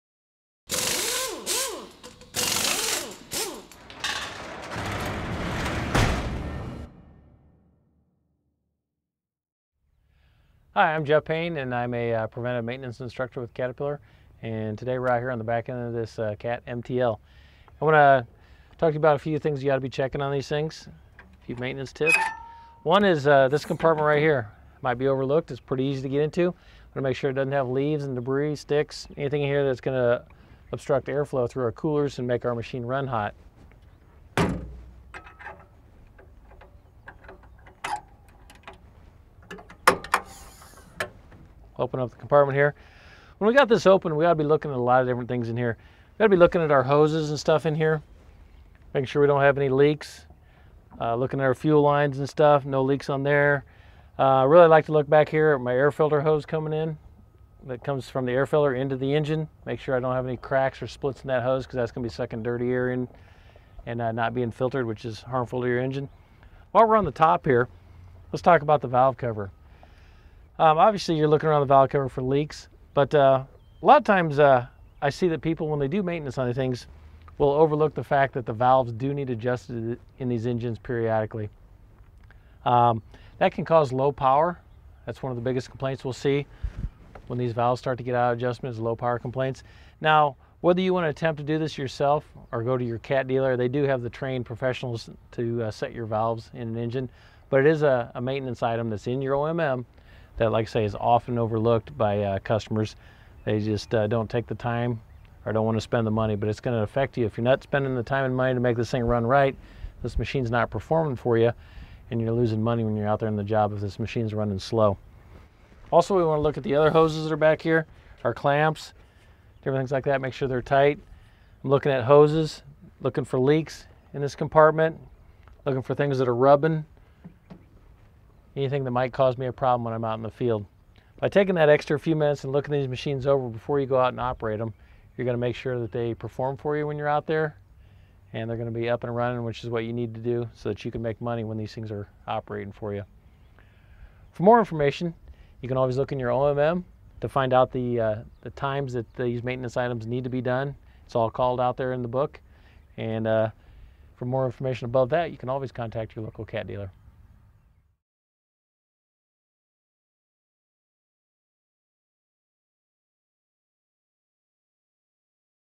Cat® Multi Terrain Loader Rear Compartment Tips
Rear compartment maintenance checks on your Cat® Multi Terrain Loader (MTL) takes just a few minutes and can save you hours of downtime "down the road." Watch this brief video and get important maintenance tips from Cat Maintenance Expert Jeff Payne.